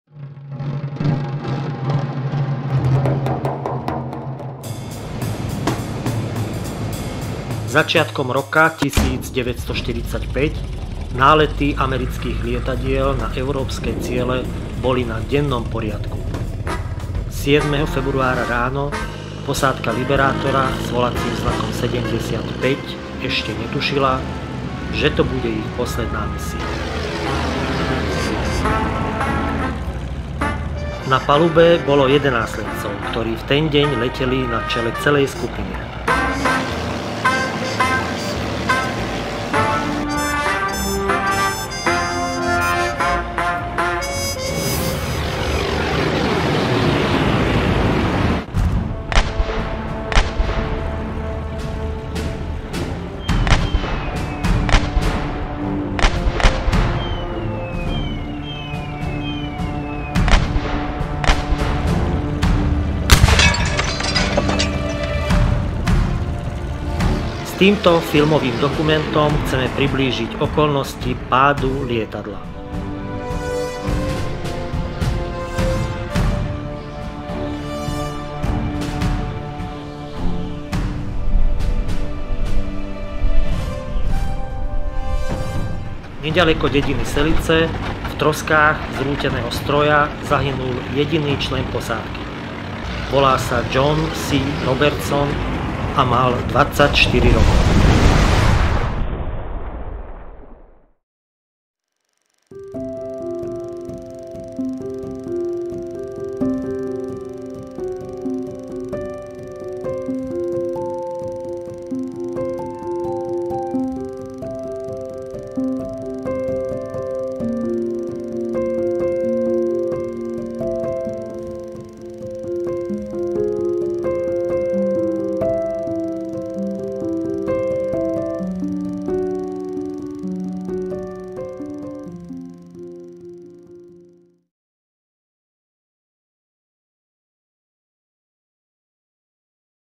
The Last Mission (Teaser)
The Last Mission of B-24 Liberator 44-49216
7.2.1945 - Magyarsók/Selice